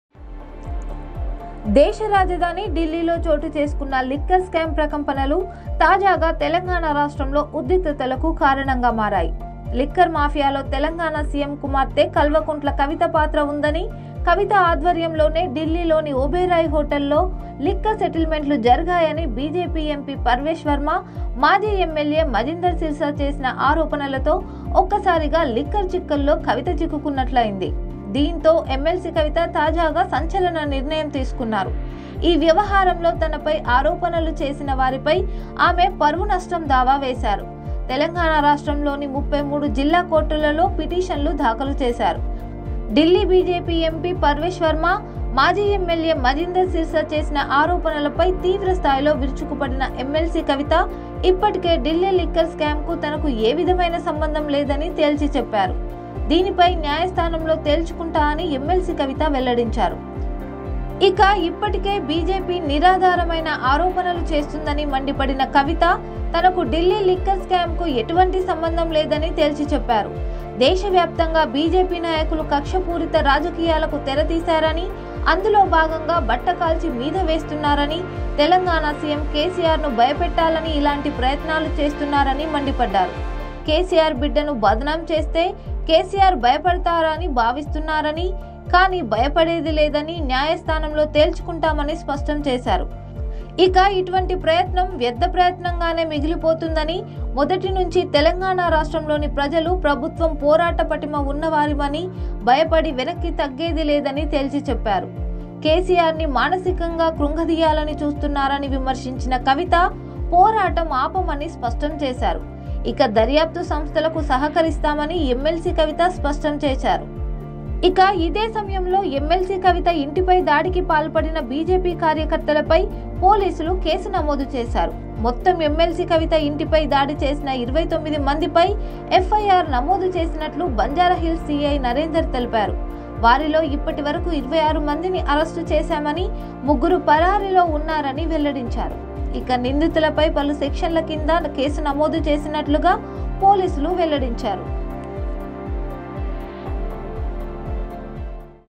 భయపడం... న్యాయస్థానంలో తేల్చుకుంటాం... కవిత *Political | Telugu OneIndia
MLC Kavitha filed defamation suit in 33 district courts of Telangana, against BJP MP Parvesh Varma and former MLA Manjinder Singh Sirsa, who accused her in the Delhi liquor scam row | దేశరాజధాని ఢిల్లీలో చోటుచేసుకున్న లిక్కర్ స్కామ్ ప్రకంపనలు తాజాగా తెలంగాణ రాష్ట్రంలో ఉద్రిక్తతలకు కారణంగా మారాయి. లిక్కర్ మాఫియాలో తెలంగాణ సీఎం కుమార్తె కల్వకుంట్ల కవిత పాత్ర ఉందని, కెసిఆర్ కుమార్తె కవిత ఆధ్వర్యంలోనే ఢిల్లీలోని ఓబెరాయ్ హోటల్లో లిక్కర్ సెటిల్మెంట్లు జరిగాయని బీజేపీ ఎంపీ పర్వేష్ వర్మ, మాజీ ఎమ్మెల్యే మంజీందర్ సిర్సా చేసిన ఆరోపణలతో ఒకసారిగా లిక్కర్ చిక్కుల్లో కవిత చిక్కుకున్నట్లు అయింది